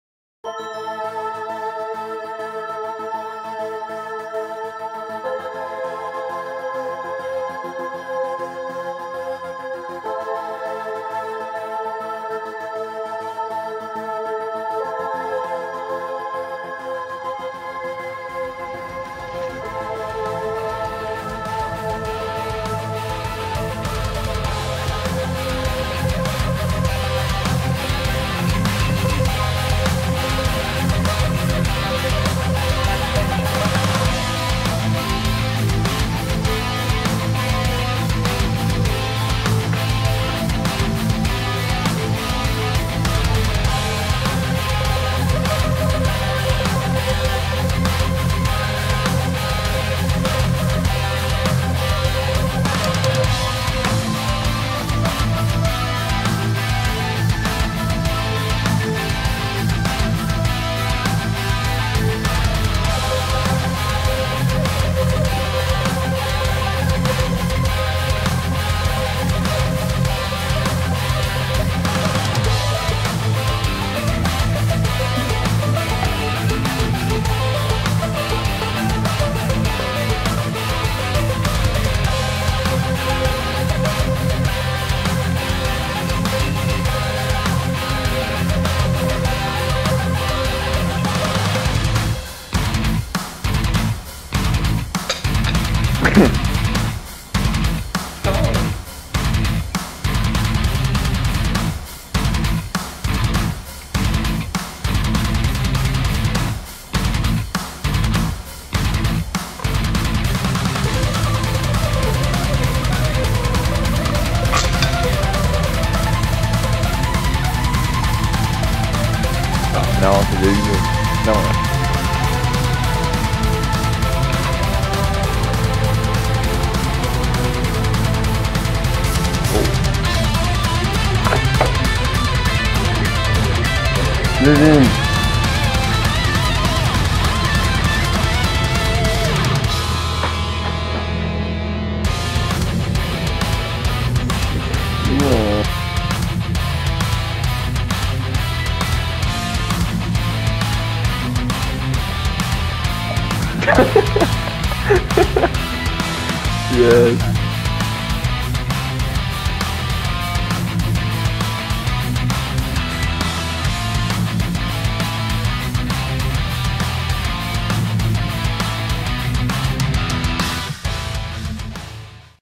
Diabolo Time
Moi qui fait du diabolo et un peu de 2diabolos.